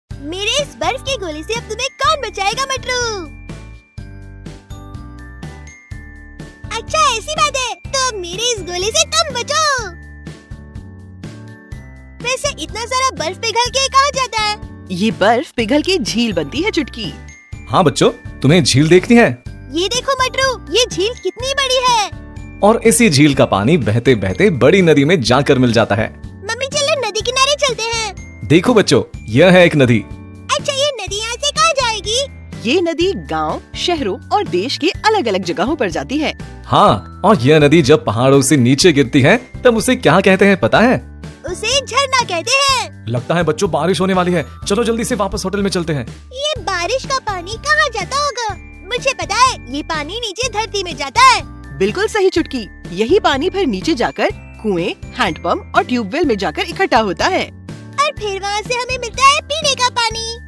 🌊 पानी कहाँ से आता है ⛲ | Sources of Water | Kids Video | Ages 3-8
इस वीडियो में, मटरू और छुटकी यात्रा पर जाते हैं और सीखते हैं कि पानी कहाँ से आता है। उनकी छुट्टियों में शामिल हों जहां बर्फ, झरने और झीलें है और पता लगाएं कि पानी के स्रोत क्या है!

In this video, Matru and Chutki go on a trip and learn what are the different sources of water. Join them on their holiday where there is snow, waterfalls, and lakes, and find out where do we get water from!